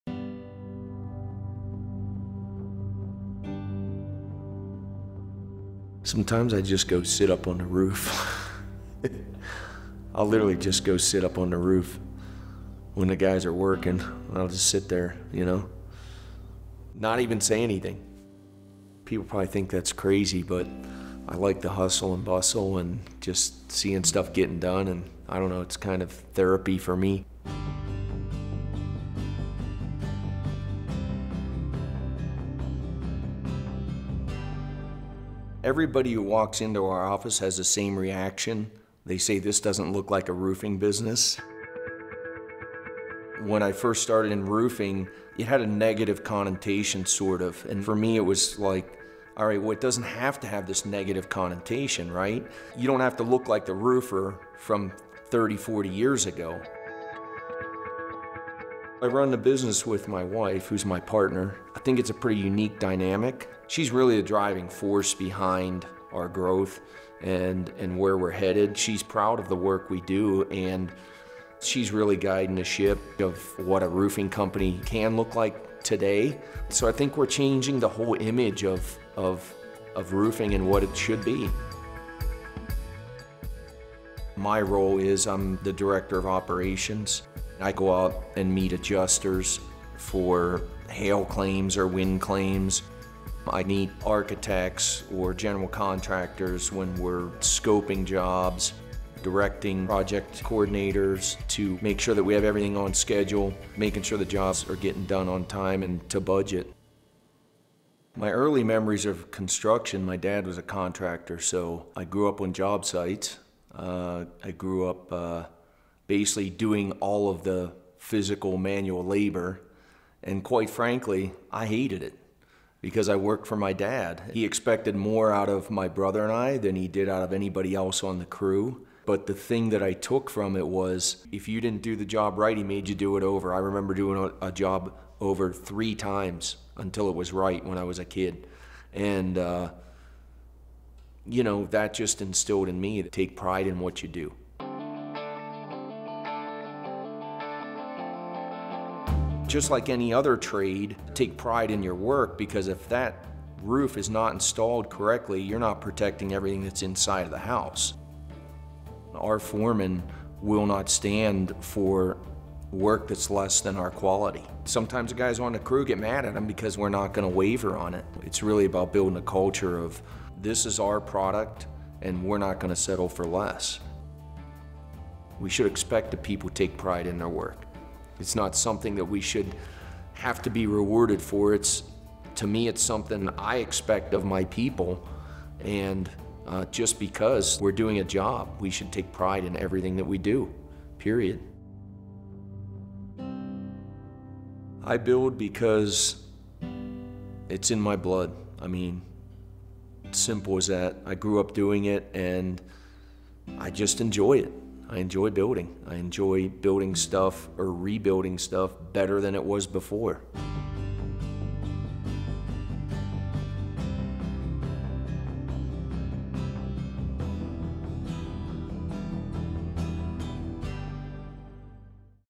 Why I Build #8 - Kelly Stitzer, Roofer
Changing the face of roofing: This third-generation contractor has an unrelenting commitment to quality and to doing jobs right the first time.